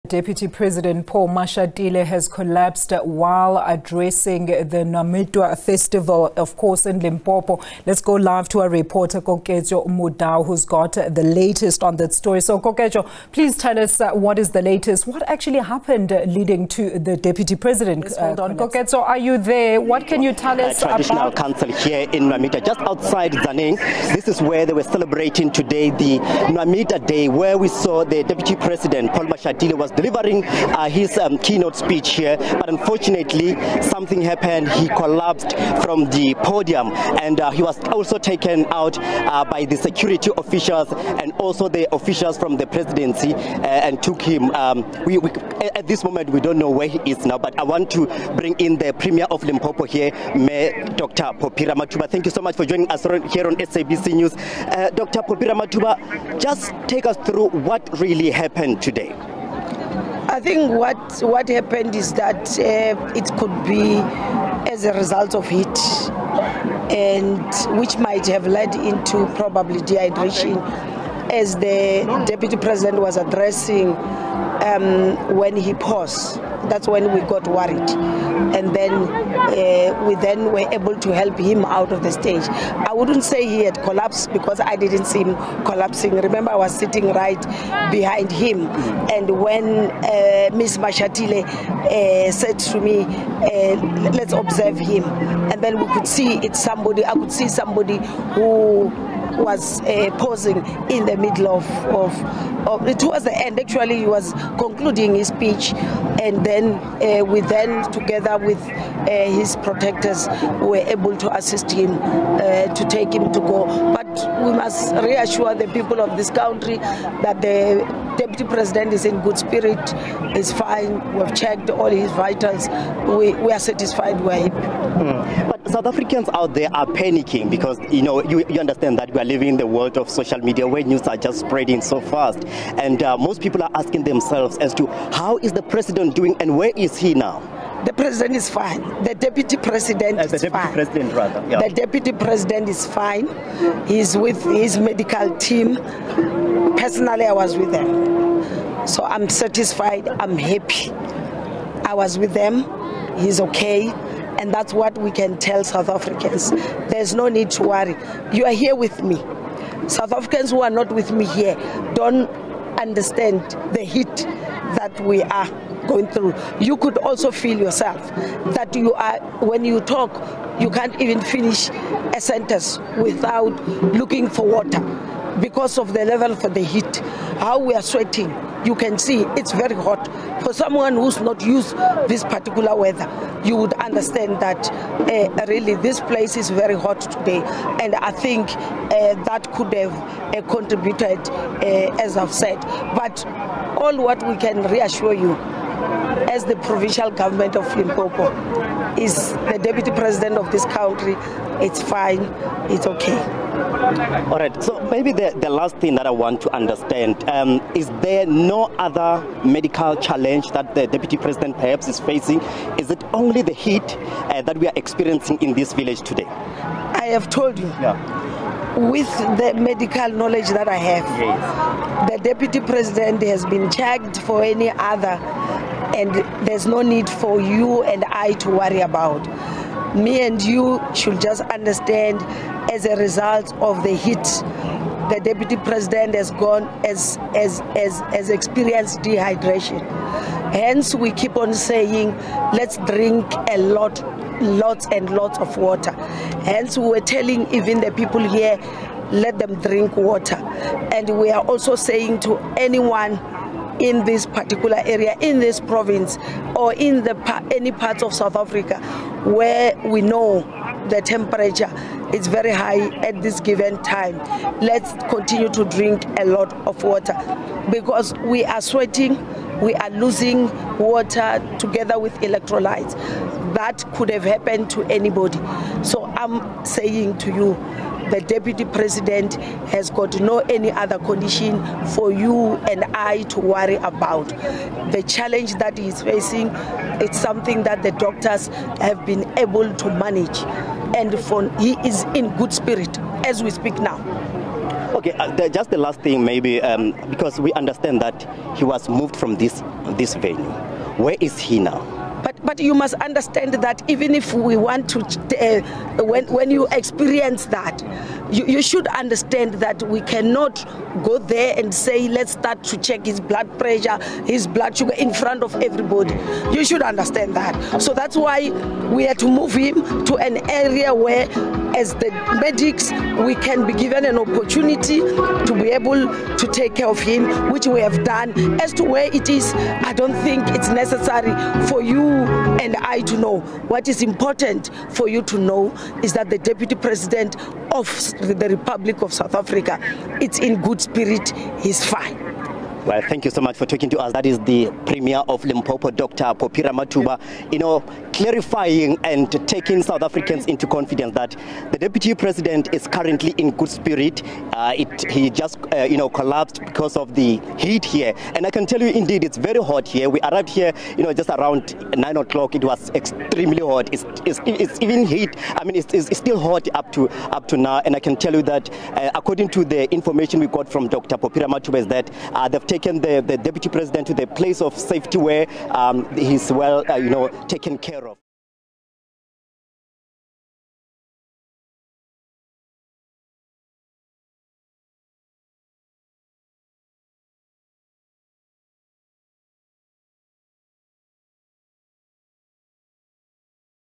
Deputy President Paul Mashatile collapses on stage in Tzaneen
Deputy President Paul Mashatile collapsed on the podium while addressing Nwamitwa Day in Tzaneen in Limpopo and was rushed to a local hospital.

Limpopo Premier Dr Phophi Ramathuba says it could have been due to dehydration.

She says the deputy president does not have any underlying medical conditions.